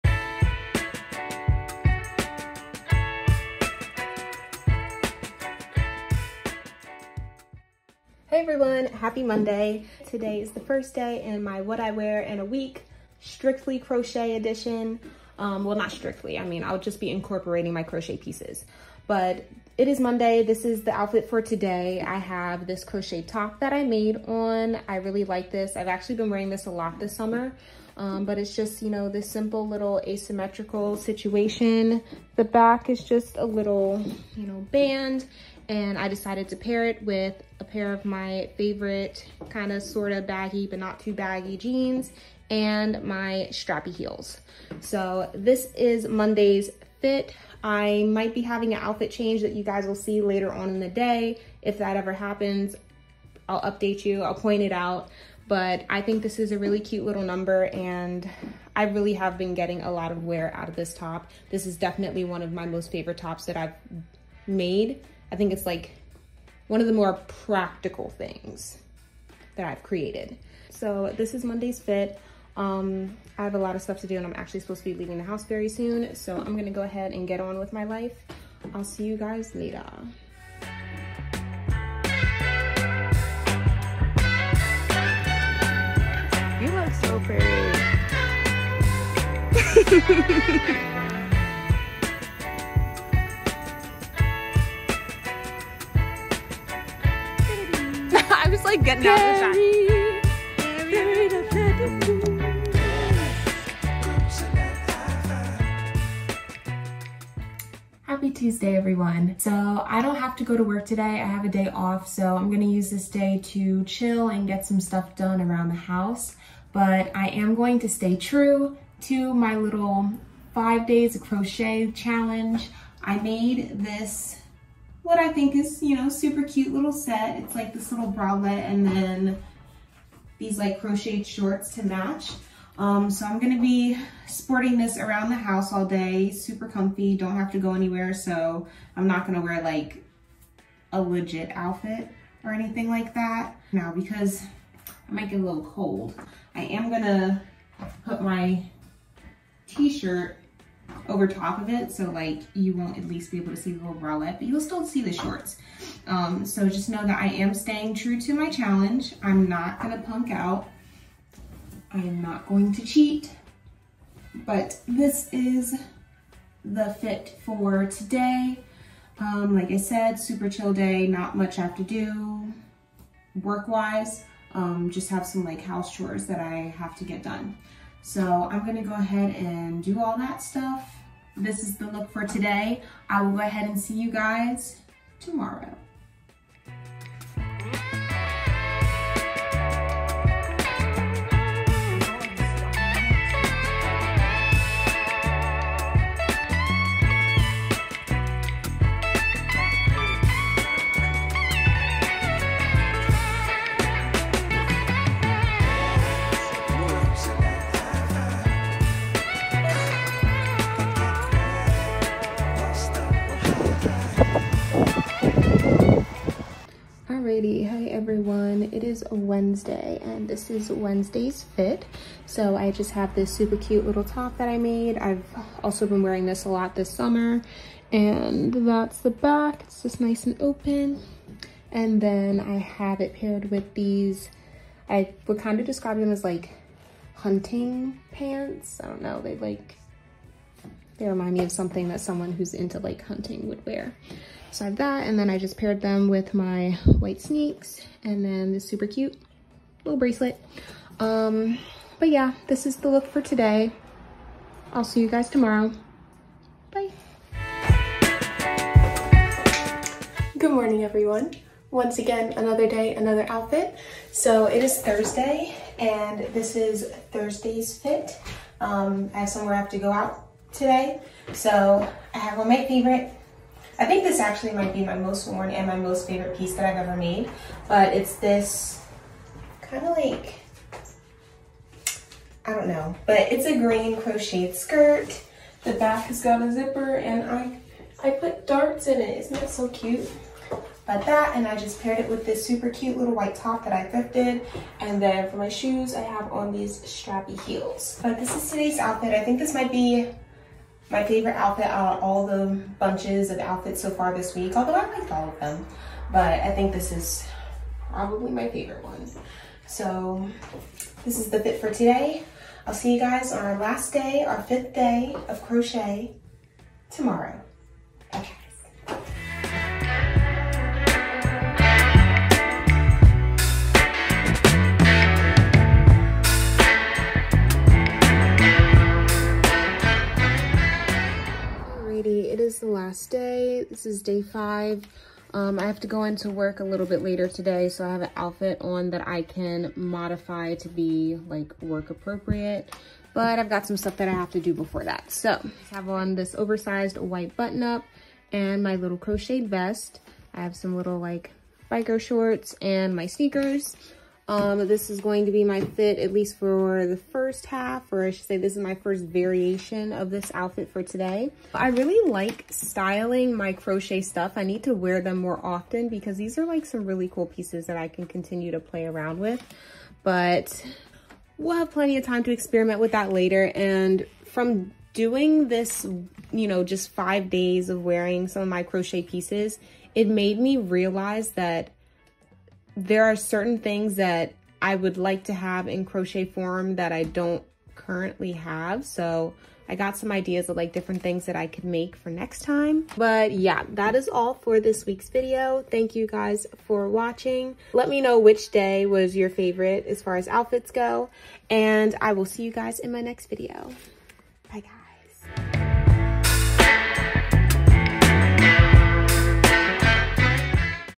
only wearing the things i've made for a week | mini vlog
Hey guys, this week I’ve challenged myself to wear at least one of my crochet projects every day for 5 days. I make so many fun things and I want to make sure I’m getting good use out of them! Hope you like the fits, let me know which is your favorite in the comments.  Also check out Ashleys channel, she’s such a sweetheart, so talented and funny!